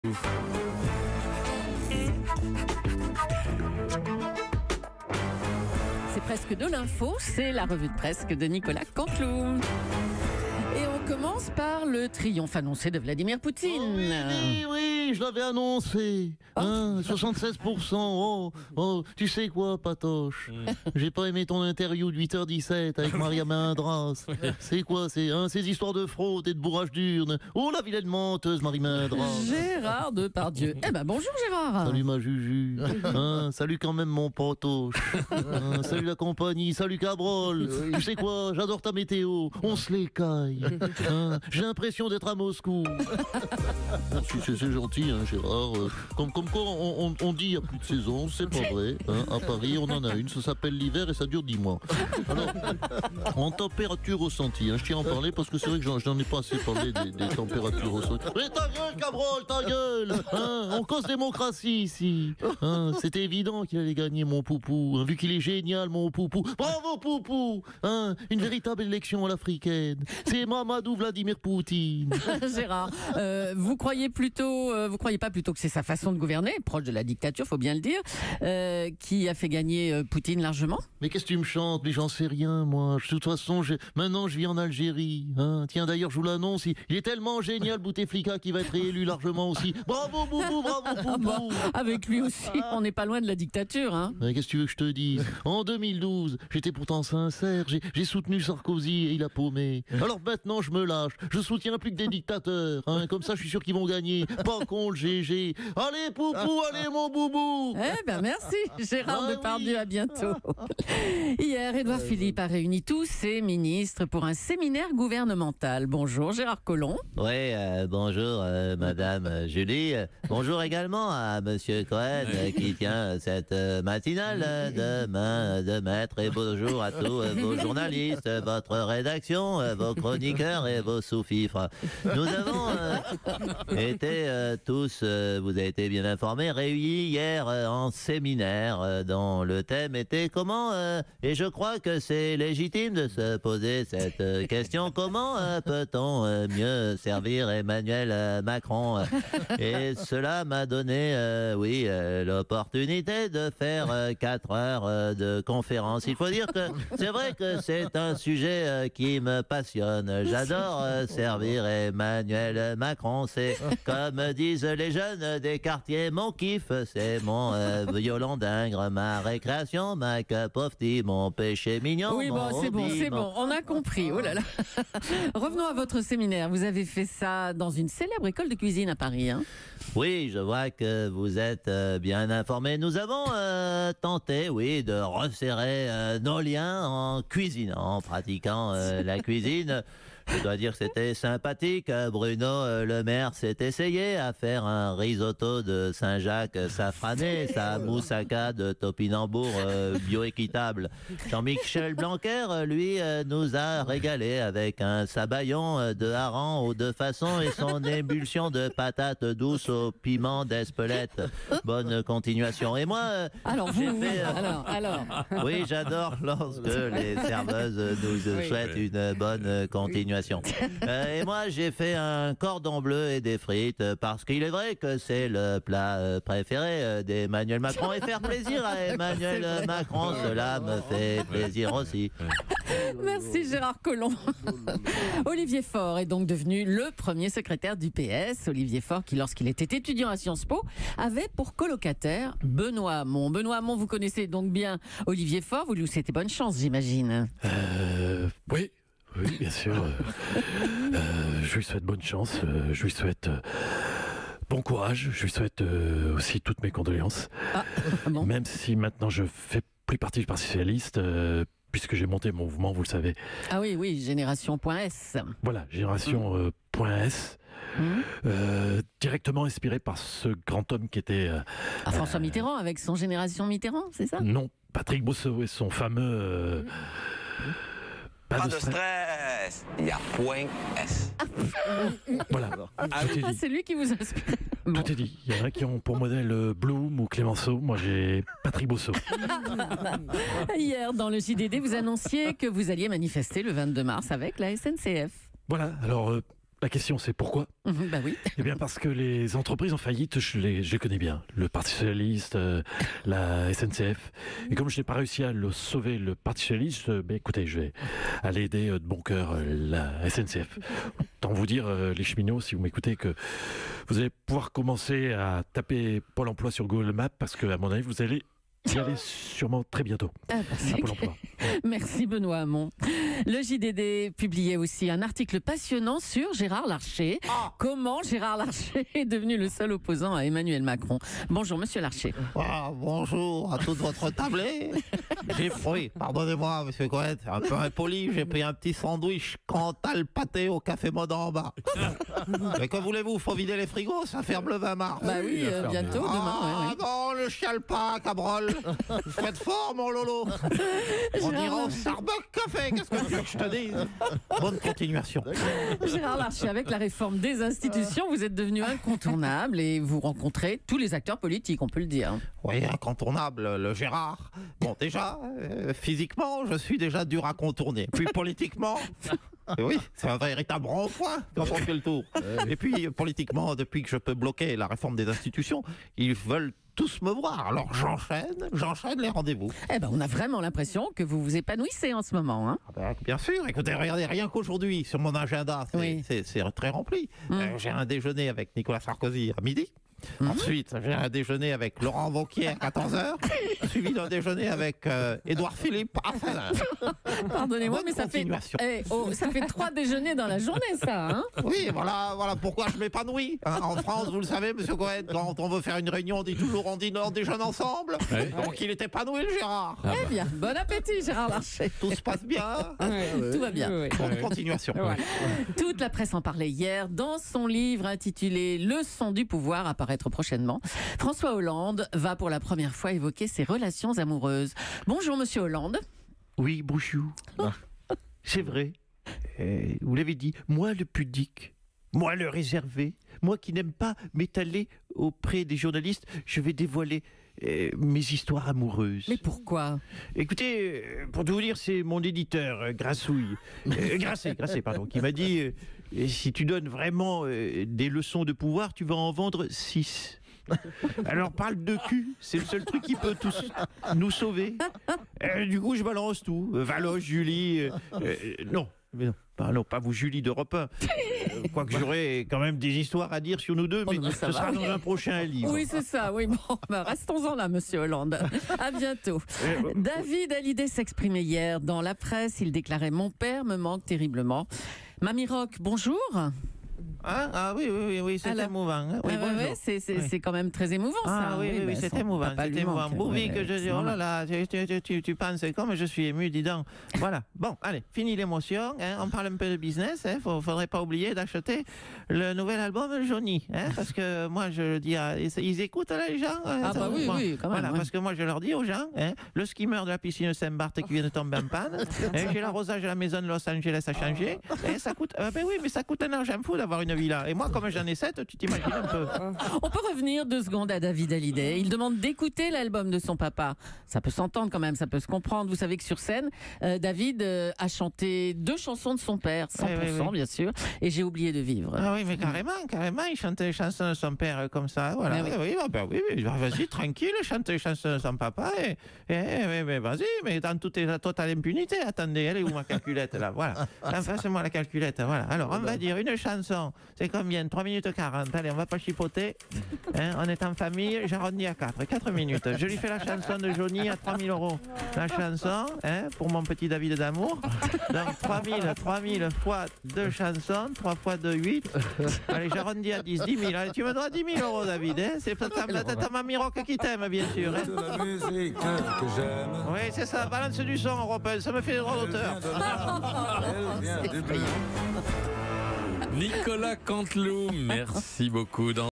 Depardieu : "Maintenant, je ne soutiens plus que les dictateurs !" (Nicolas Canteloup)
Gérard Depardieu se réjouit de la victoire de son "Poupou" : Vladimir Poutine !